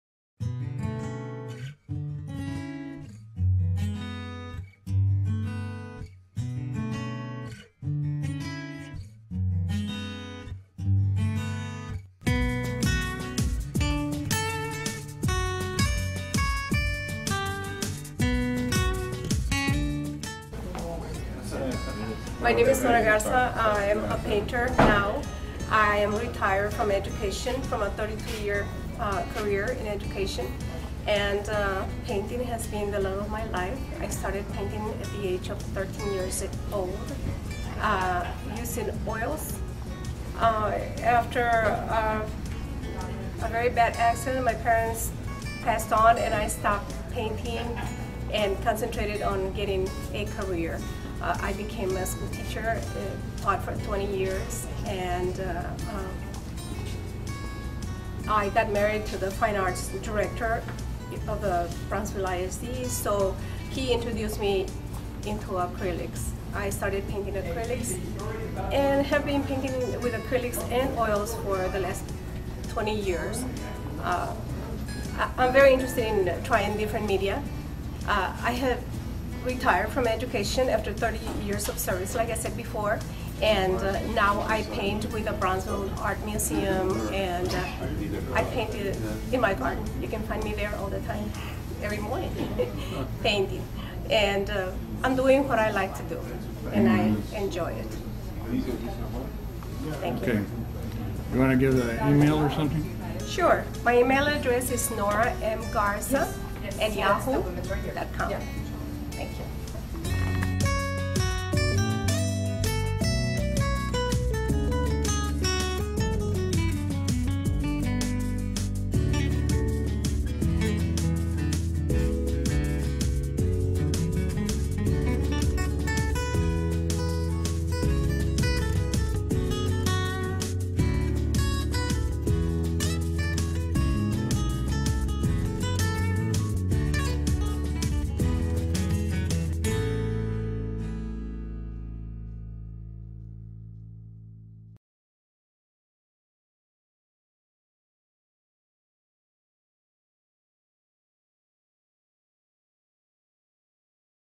Nora M. Garza at Authors_ Night at Narciso Martinez Cultural Arts Center
Interview with Rio Grande Valley Artist Nora M. Garza